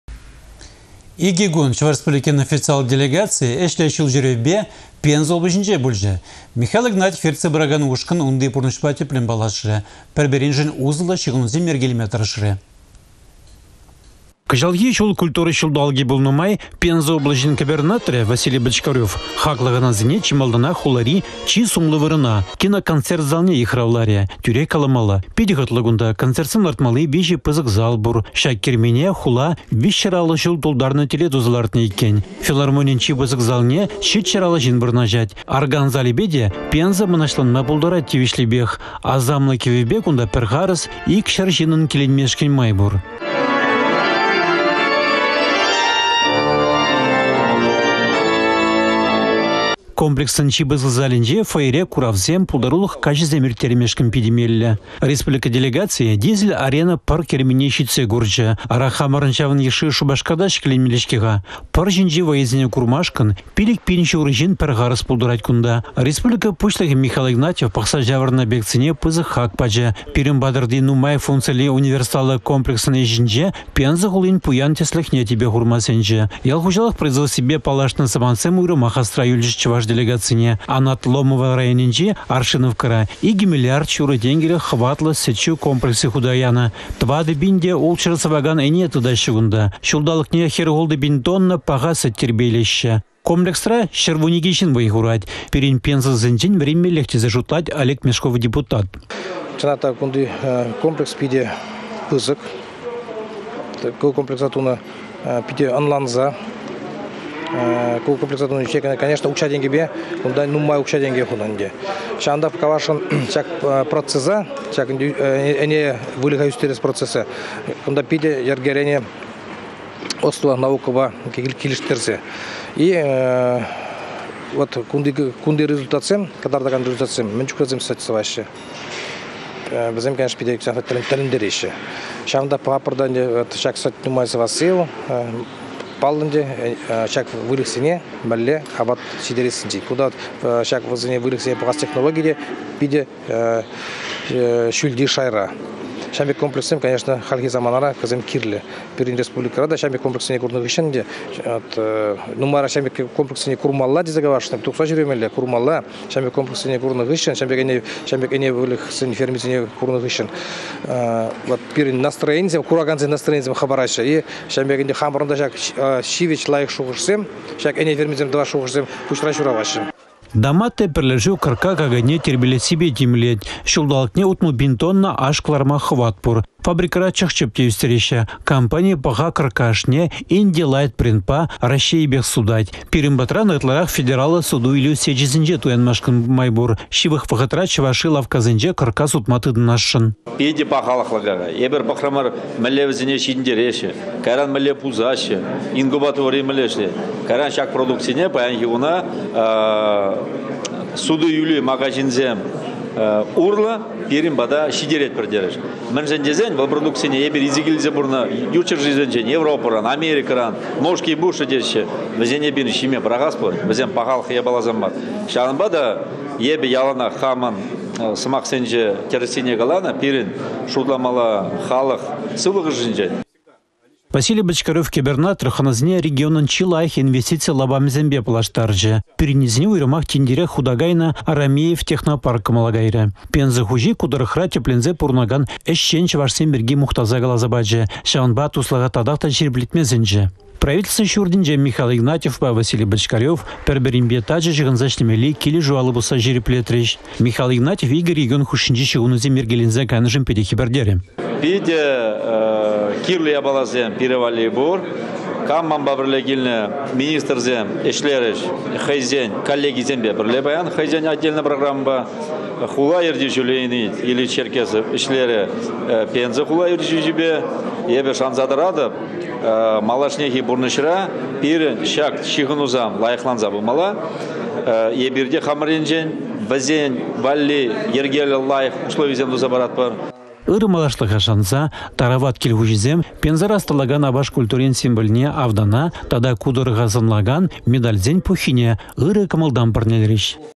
Чăваш Ен Пензăпа туслашрĕ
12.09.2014
Икĕ кун Чăваш республикин официаллă делегацийĕ ĕçлĕ çул çÿревпе Пенза облаçĕнче пулчĕ. Михаил Игнатьев ертсе пыракан ушкăн унти пурнăçпа тĕплĕн паллашрĕ, пĕр-пĕриншĕн усăллă çыхăнусем йĕркелеме тăрăшрĕ.